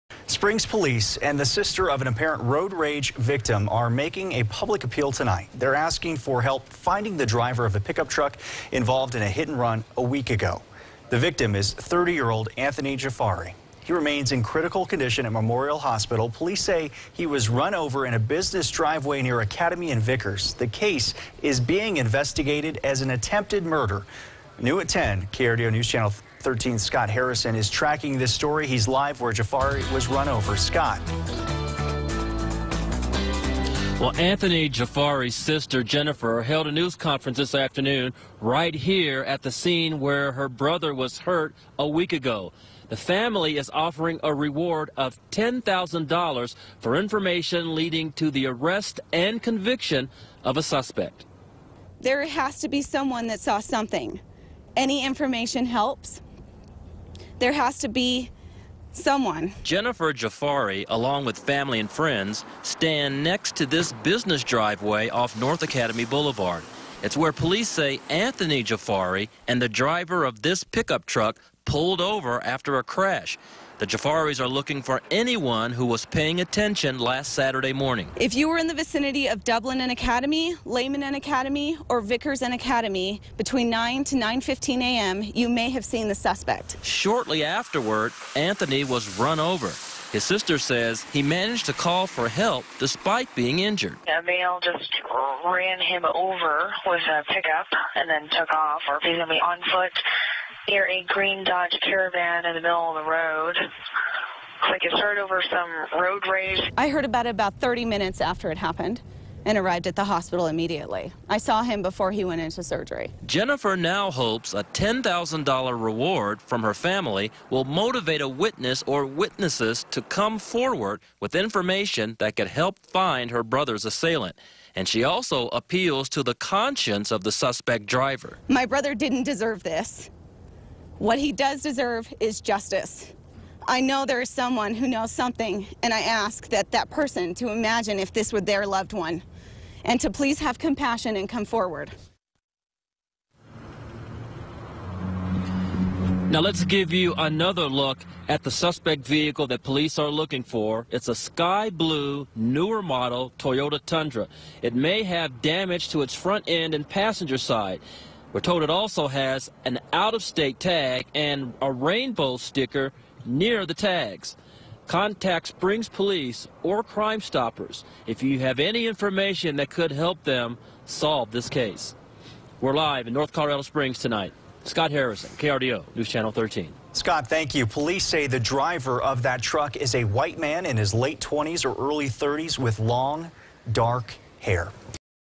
Reward Offered in Road Rage Case
Family of victim announces $10,000 reward for information leading to a suspect's arrest and conviction.